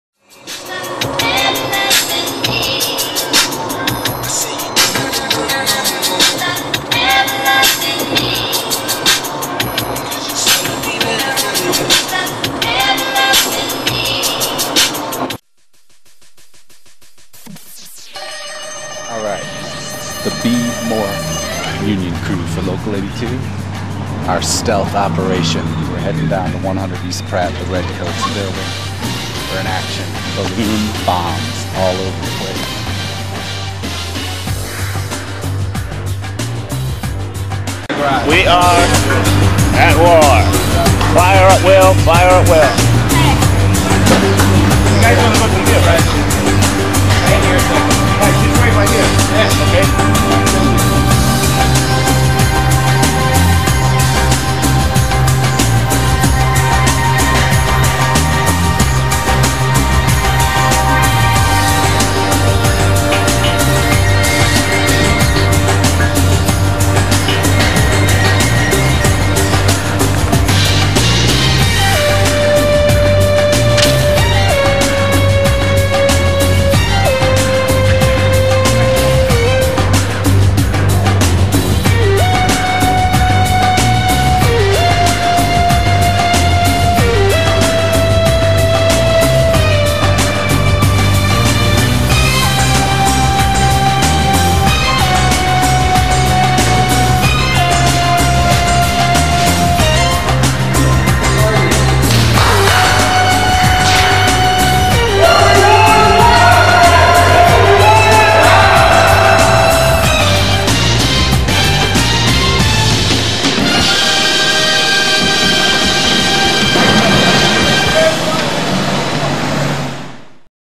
SEIU Justice for Janitors Campaign Baltimore, MD - Balloon Bombs
2001 East Coast Justice for Janitors Campaign Local 82 Baltimore, MD fighting for livable wages, better benefits and health insurance.  Filmed and edited by Norman Navarro SEIU International Union Organizer.